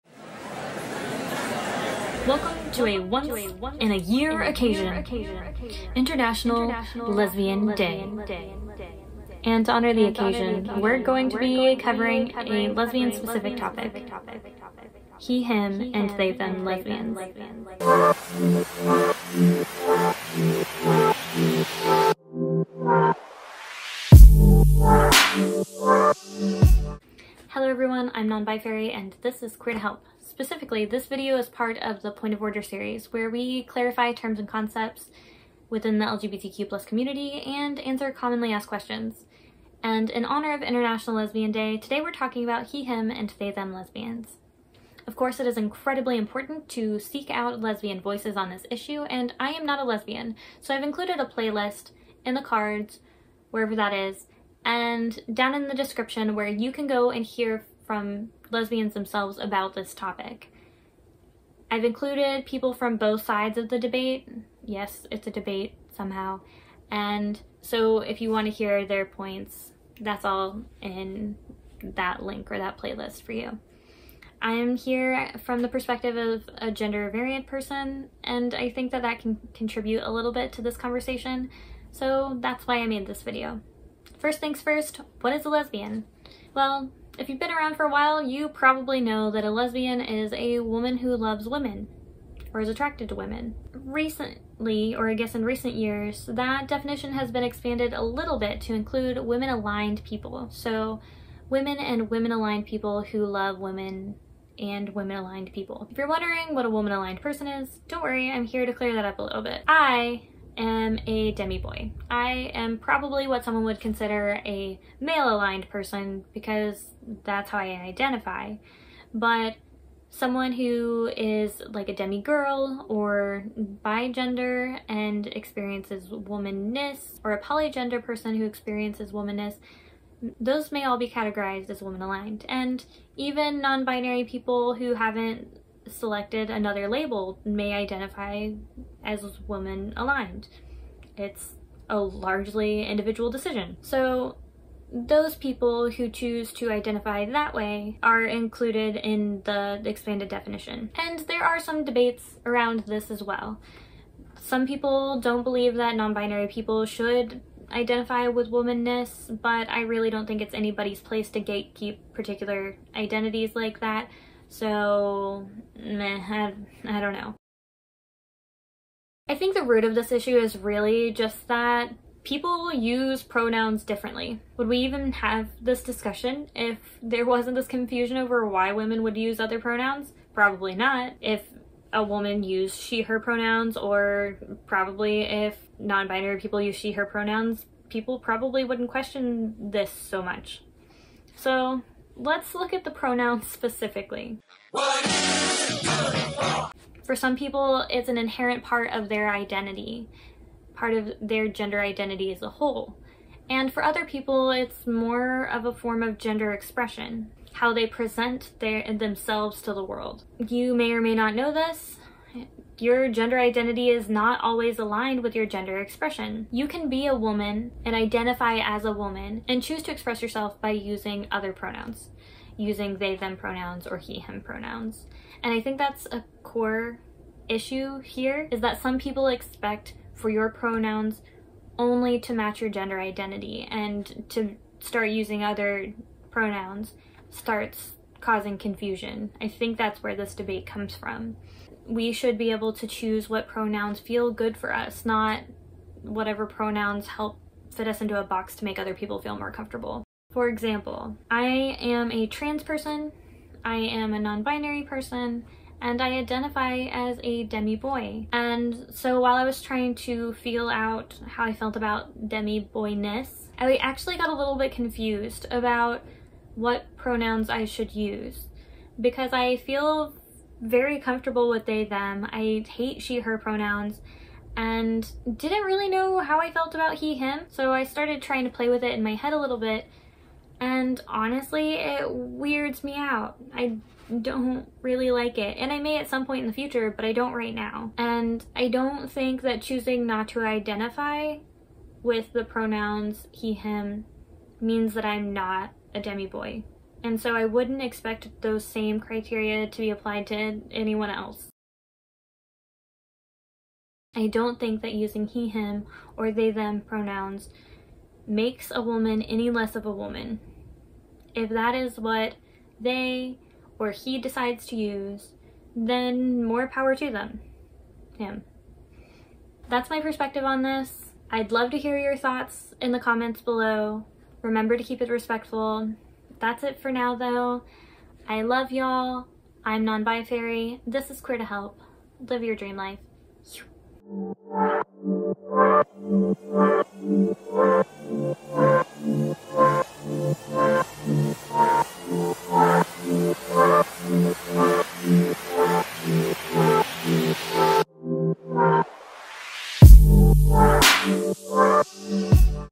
He/Him and They/Them Lesbians? | Point of Order
He/Him and They/Them lesbians are somehow the subject of debate. So, to celebrate International Lesbian Day, let's tackle this discussion from my perspective as a gender variant person. I think the root of this problem is how we use pronouns. What do you think?

Chapters
0:00 Intro
0:44 Playlist Explanation
1:23 What is a LESBIAN?
2:47 What's the issue?
3:14 What good are pronouns?
4:10 My experience with pronoun confusion
5:01 The point?